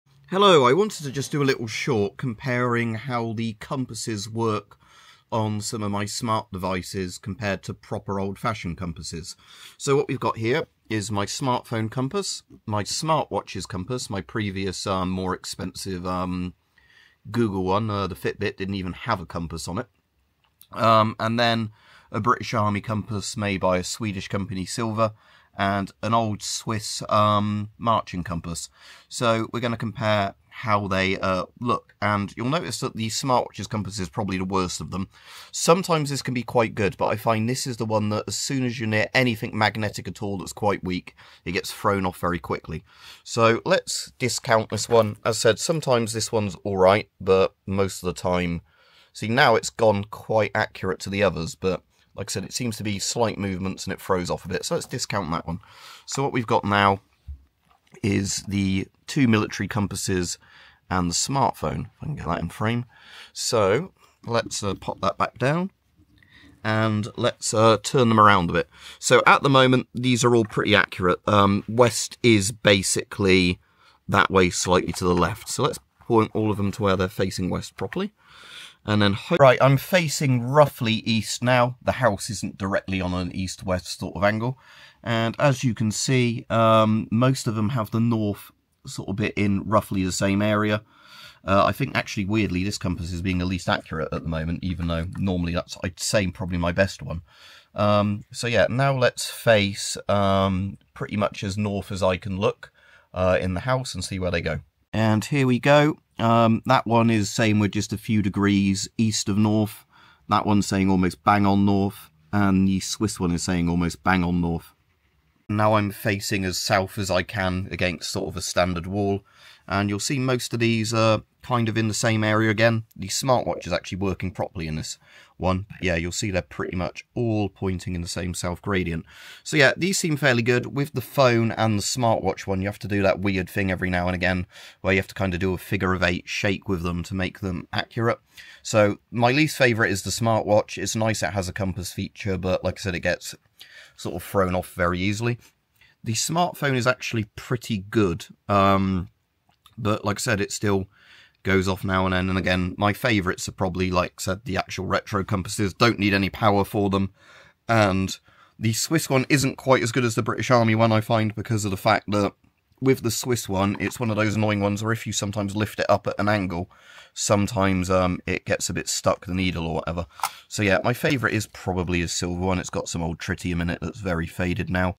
Testing Smart compasses to retro magnetic ones
Testing how accurate smart compasses on watches and phones are compared to the old magnetic ones.